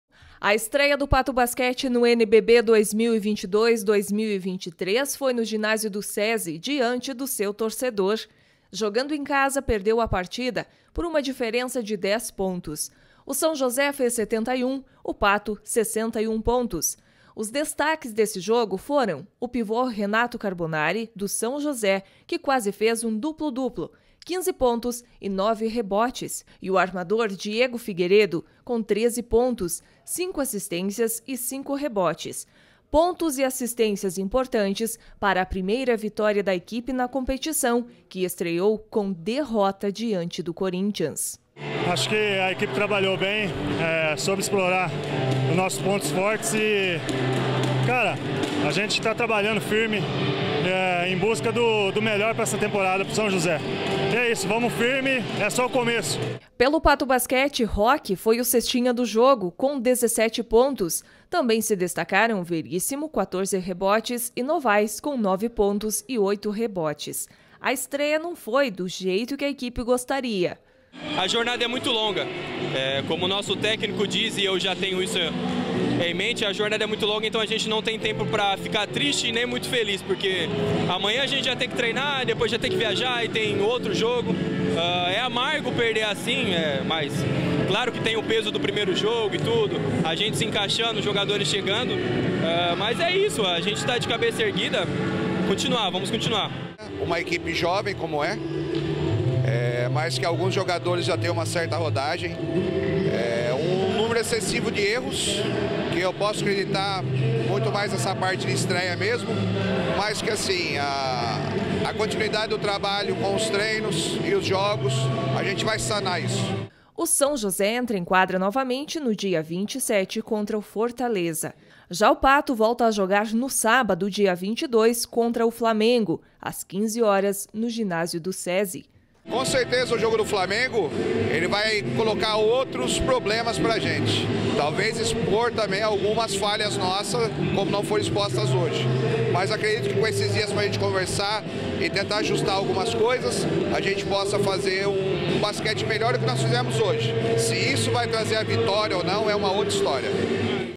Na estreia no NBB, Pato Basquete é derrotado pelo São José
Esporte: São José Vence o Pato Basquete pelo placar de 71 a 61. Foi a primeira vitória do São José na competição, após derrota na estreia para o Corinthians. No caso do Pato, era a estreia no NBB.